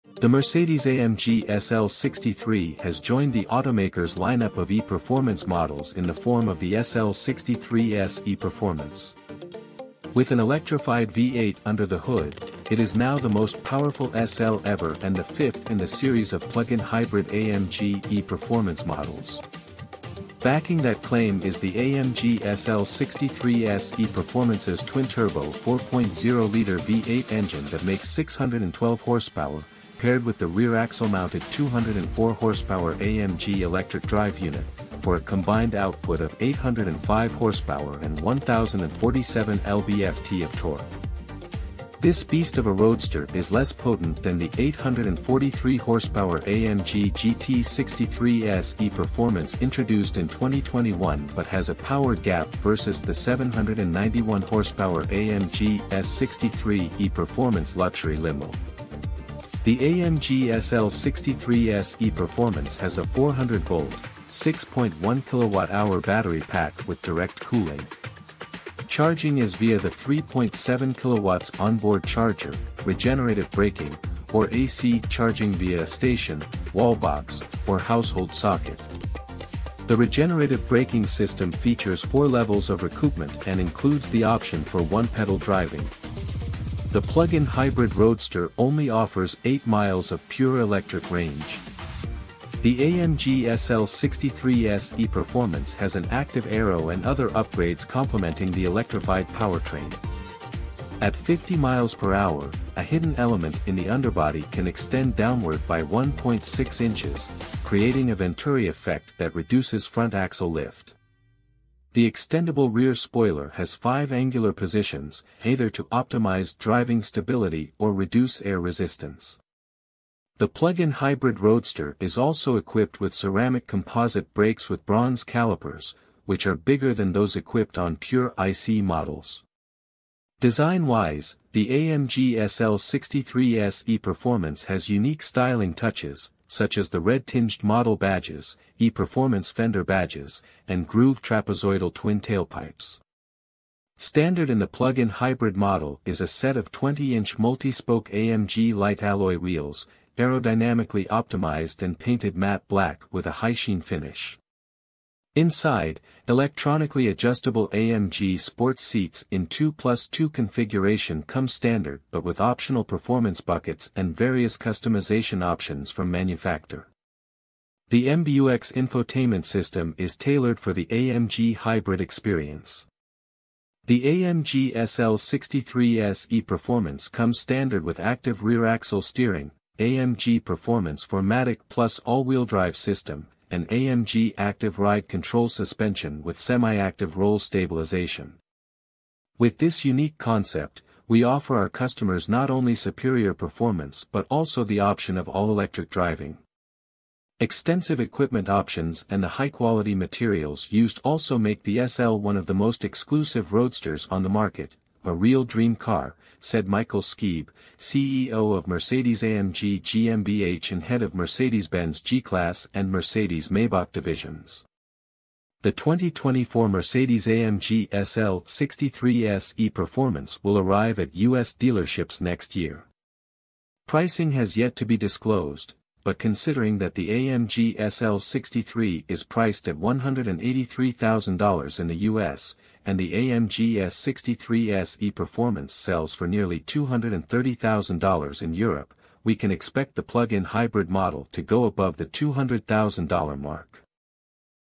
2024 Mercedes-AMG SL63 S E Performance Debuts With 805-HP Electrified V8 Power
It's the most powerful SL yet.

Plug-in hybrid twin-turbo 4.0-liter V8 produces up to 805 horsepower and 1,047 lb-ft
0-60 mph in 2.8 seconds, 196 mph top speed
8 miles of pure electric driving
The Mercedes-AMG SL63 has joined the automaker's lineup of E Performance models in the form of the SL63 S E Performance. With an electrified V8 under the hood, it is now the most powerful SL ever and the fifth in the series of plugin hybrid AMG E Performance models.

Backing that claim is the AMG SL63 S E Performance's twin-turbo 4.0-liter V8 engine that makes 612 horsepower, paired with the rear-axle-mounted 204-hp AMG Electric Drive Unit, for a combined output of 805 horsepower and 1,047 lb-ft of torque. This beast of a roadster is less potent than the 843-hp AMG GT 63 S E Performance introduced in 2021 but has a power gap versus the 791-hp AMG S63 E Performance luxury limo.